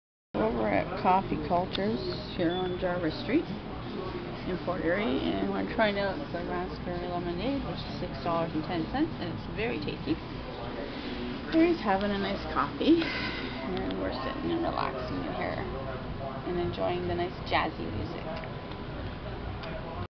Raspberry Lemonade Smoothee Coffee Culture 26 Jarvis St Fort Erie Ont 905 871 2228
We were out filming Ben's treasures & more consignment retail shop store and stopped in to enjoy TEA TIME at Coffee Culture.  I enjoyed a new raspberry lemonade smoothee with a raisin butter tart (home made, fresh baked goods) and had a nice rest before continuing our shopping trip.

We bought tea cups and mugs on this trip (15 of them) for $10 at the two shops (st vincents has some as well).

I needed them for my christmas party 2013.
correction it's $6.10 for both of them.. harry told me it cost 6.10 without clarifying that it includes the $1.75 small coffee, i think the drinks around $ 3.95?